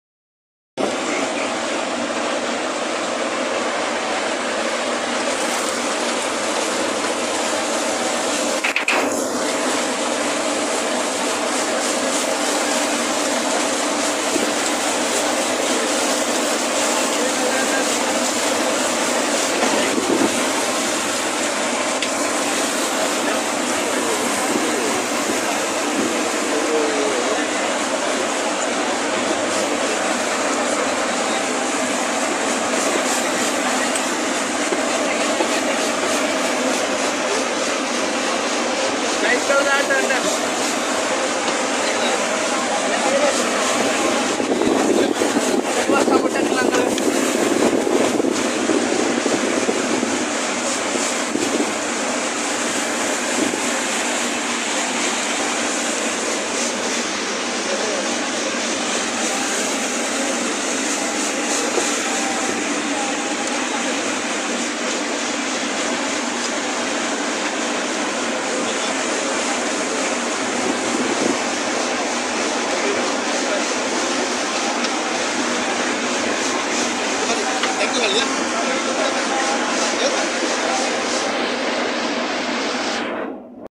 නැංගුරම් ලන යාතුාවක නොපෙනෙන ශක්තිය
ඩෝරා යාත්‍රාවක් ගොඩබිමට ආසන්න වනවිට සිදු කෙරෙන ක්‍රියාවලිය